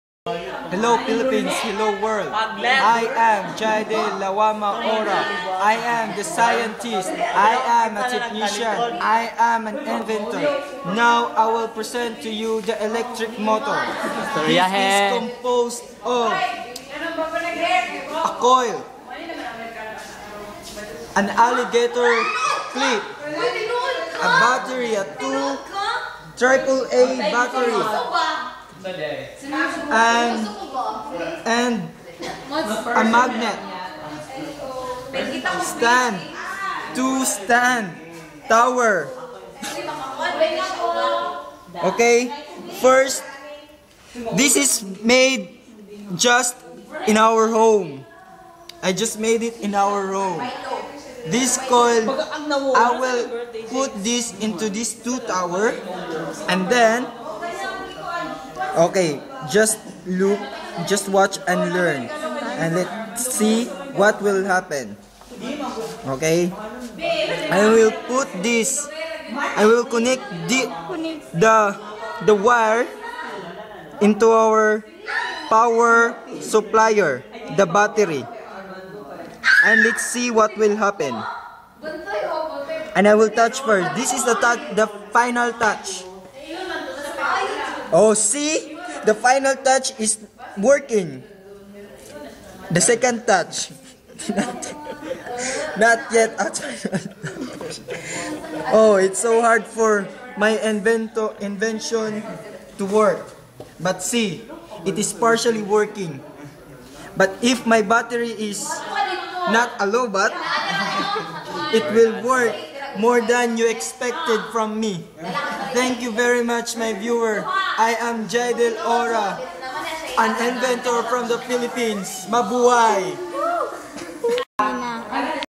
a great home-made invention
this was invented by abegail bolonos forour physics a home-made electric motor